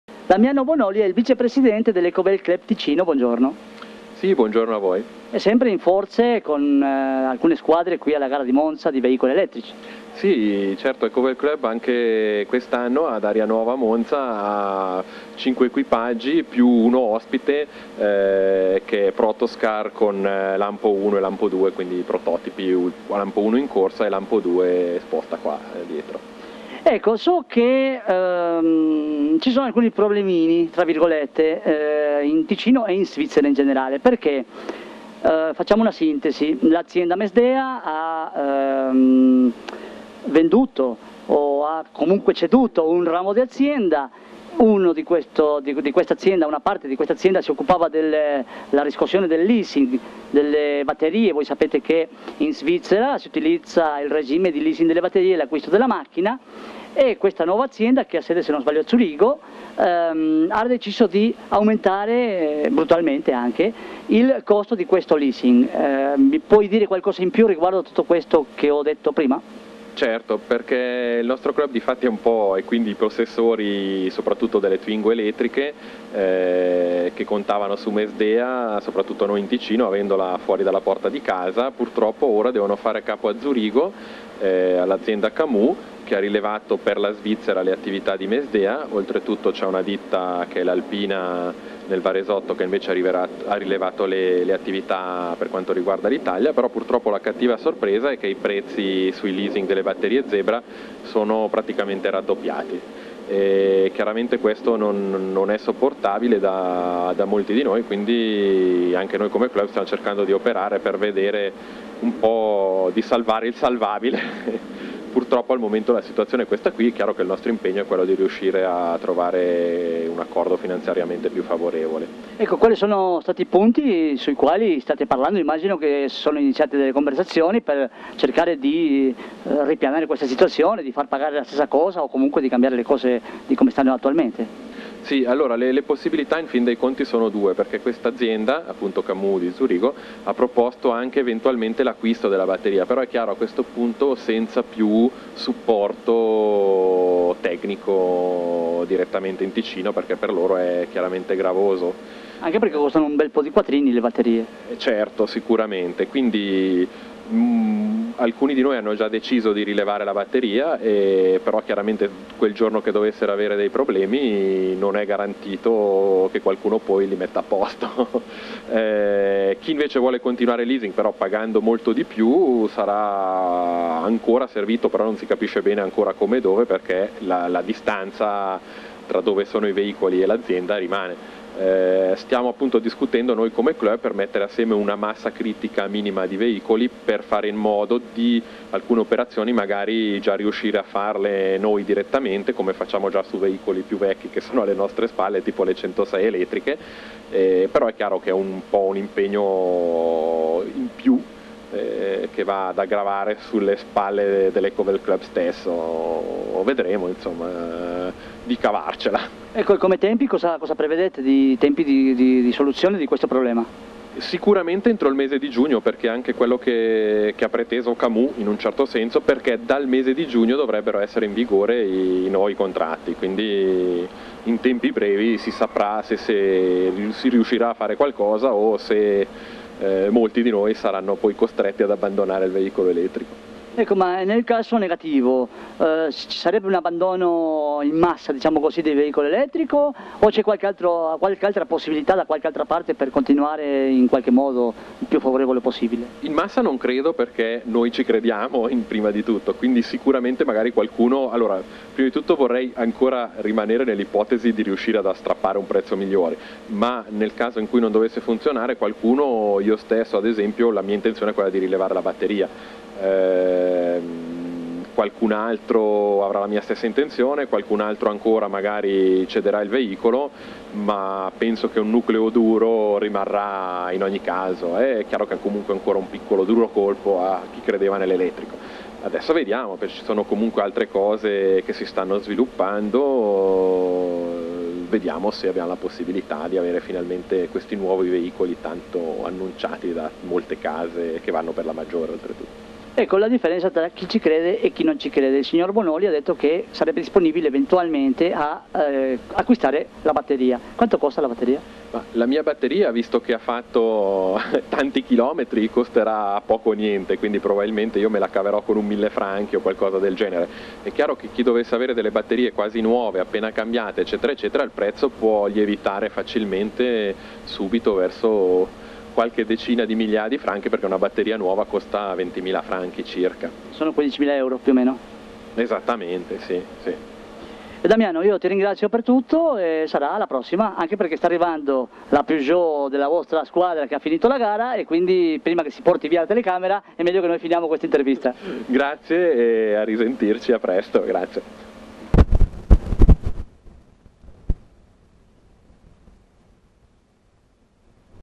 EcoVel Club Svizzera - Intervista Damiano Bonoli
Intervista a Damiano Bonoli, Vice Presidente del EcoVel Club Ticino, Svizzera, che ha partecipato alla competizione che si è svolta in occasione di Aria Nuova che si è tenuta a Monza.
Nell'intervista segnala i problemi a cui vanno incontro gli utenti svizzeri dei veicoli elettrici che prima si appoggiavano a MES DEA.
Ora l'azienda ha ceduto il ramo del settore delle batterie ad un'altra azienda di Zurigo che ha decisi di aumentare il canone di noleggio batterie.